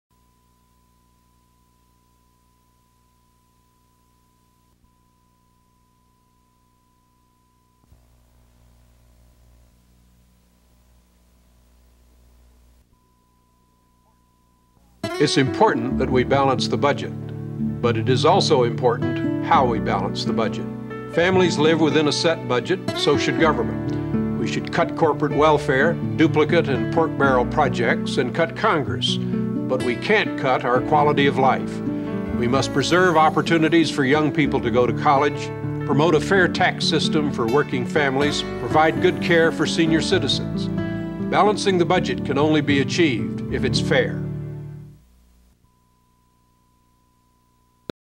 Lee H. Hamilton [D-IN] 1996 Campaign Ad "Priorities B"
1996 United States House of Representatives Offices and Issue Campaign advertisement for Lee Herbert Hamilton [Democratic]. Candidate talks about balancing the budget and the fair way to do this.

Creator: Fenn, King, Murphy, and Putnam

Location: Indiana, United States

Format: 1” videotape; video/mp4

Subjects: political campaign; Indiana elections; government; Budget; Reform; Education

Contact The Carl Albert Congressional Research and Studies Center to purchase a non-watermarked high resolution digital file‪.